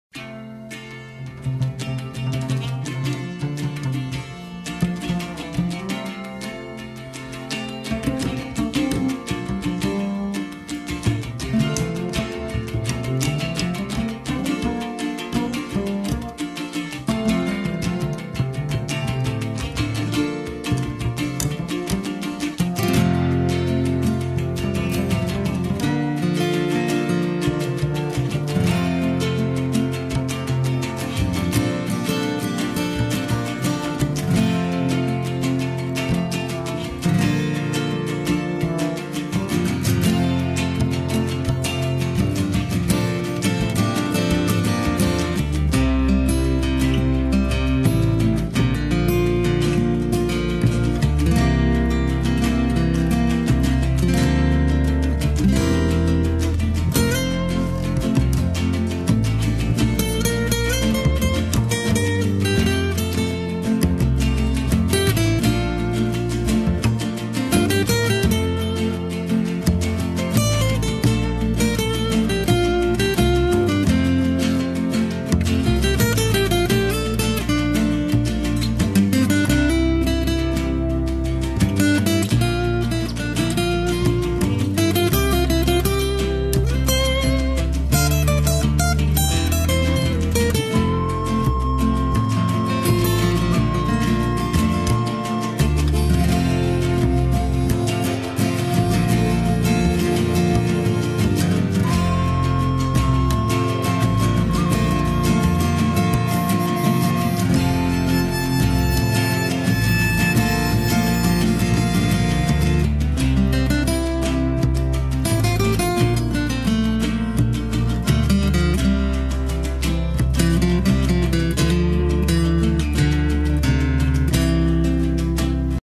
Michael Clark Arts 2011 part 2
Australian Boulder Opal Sculptures lovingly hand carved by artist Michael Clark